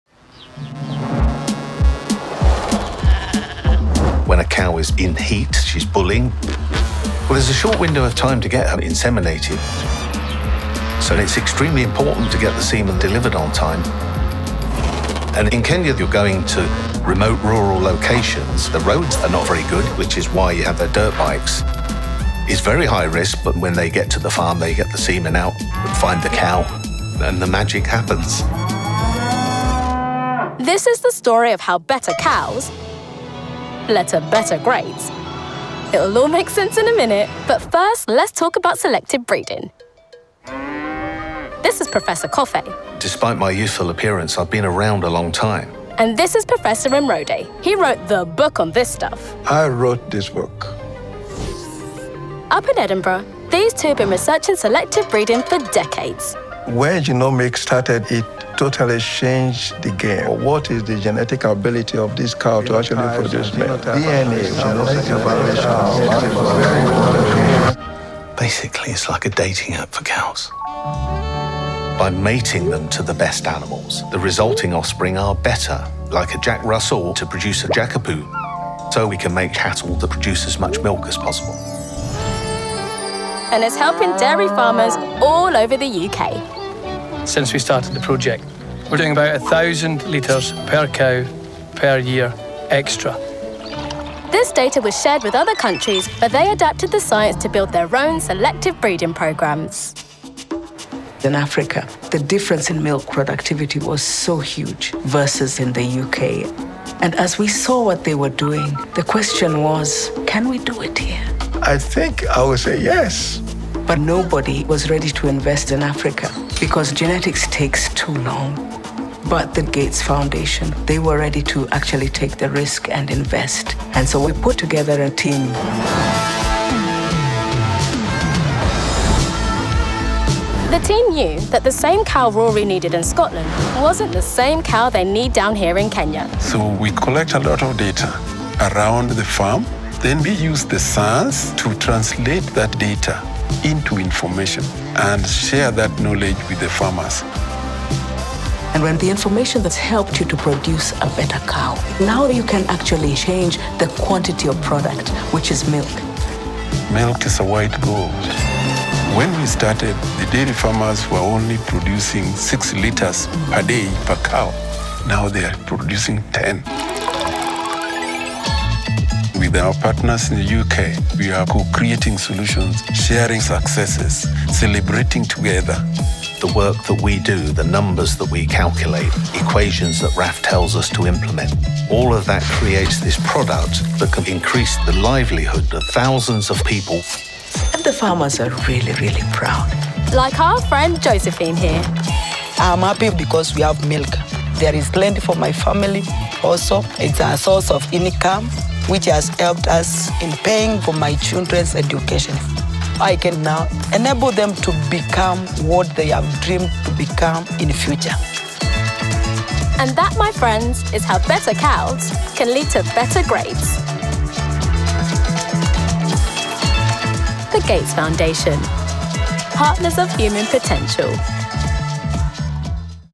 Better Cows, Better Grades | Partners of Human Potential
When you get the right partners together, the potential is endless.

When scientists in the UK and Kenya join forces to produce higher-yield dairy cows, it doesn’t just mean more milk. It means a thriving local economy, nutrition for kids, and funding for their education.